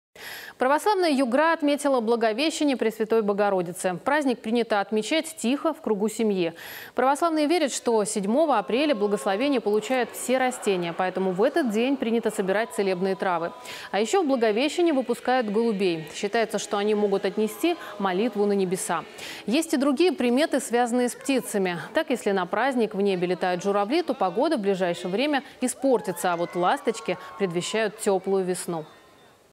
Православные югорчане отметили Благовещение Пресвятой Богородицы
Православная Югра отметила Благовещение Пресвятой Богородицы. Праздник принято отмечать тихо в кругу семьи.